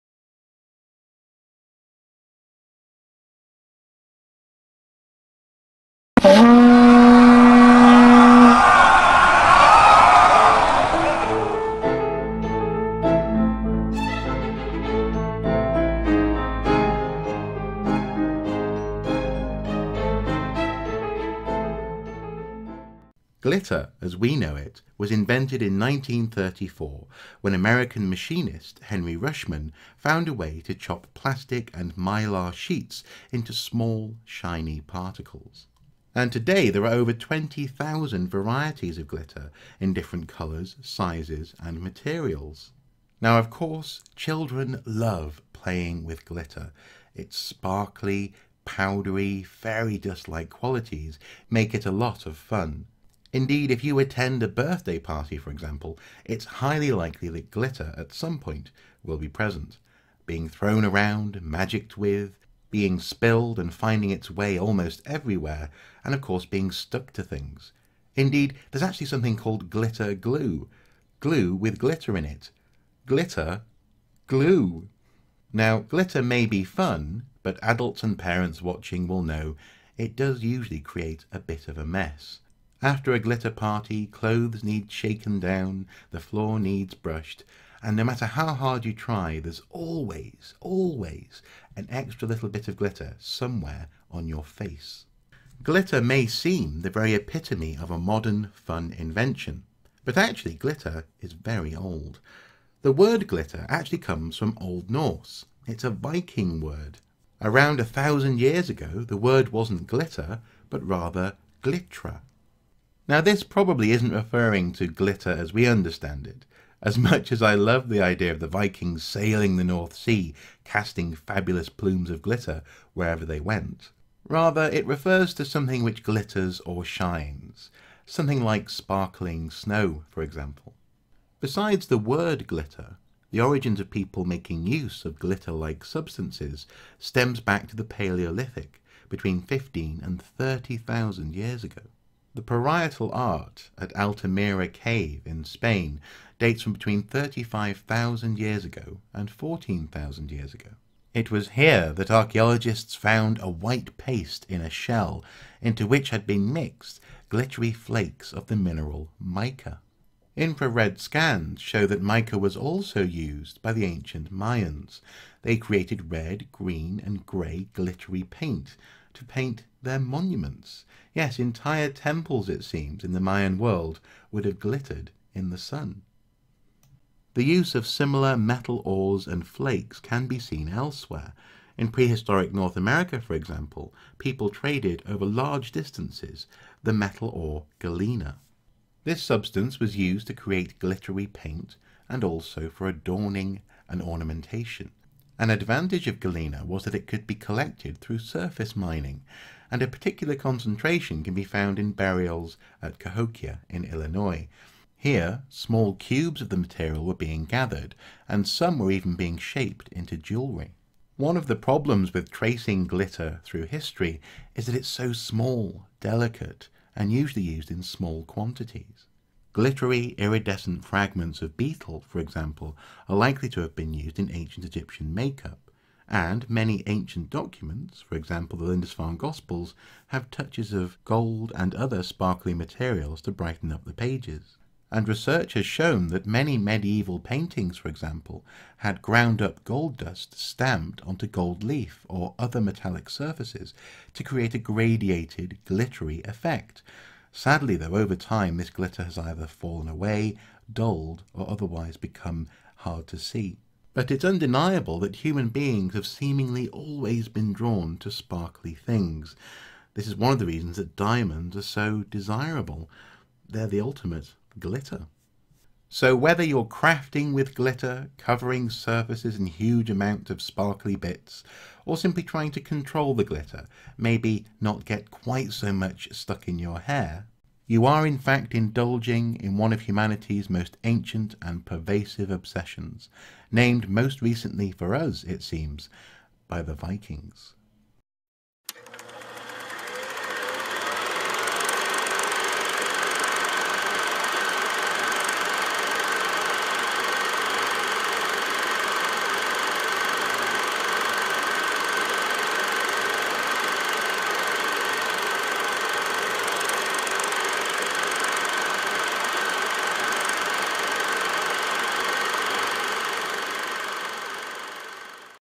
What is Glitter? : Hidden Histories
Welcome to Hidden Histories. In this series, we take a closer look at the world around us and explore the hidden depths of our shared history.

Today we take a closer look at glitter! What is glitter?